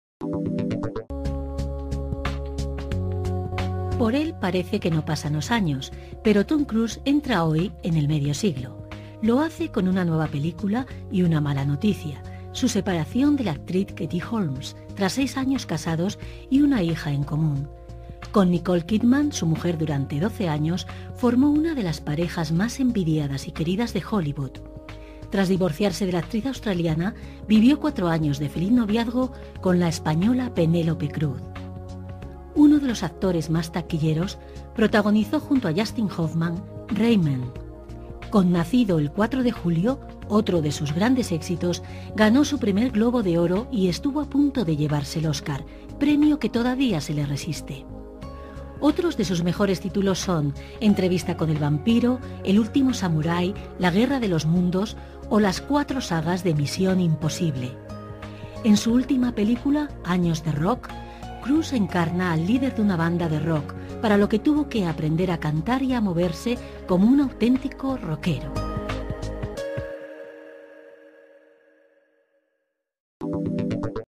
El actor norteamericano Tom Cruise cumple cincuenta años
Madrid, 3 jul (EFE).- Por él  parece que no pasan los años, pero Tom Cruise entra hoy en el medio siglo, y lo hace con una nueva película y una mala noticia: su separación de la actriz Katie Holmes, tras seis años casados y una hija en común.

Con Nicole Kidman, su mujer durante 12 años, formó una de las parejas más envidiadas y queridas de Hollywood. Tras divorciarse de la actriz australiana, vivió 4 años de feliz noviazgo con la española Penélope Cruz.

En su última película,"Años de rock", Cruise encarna al líder de una banda de rock en el ocaso de su carrera, para lo que tuvo que aprender a cantar y a moverse como un auténtico rockero.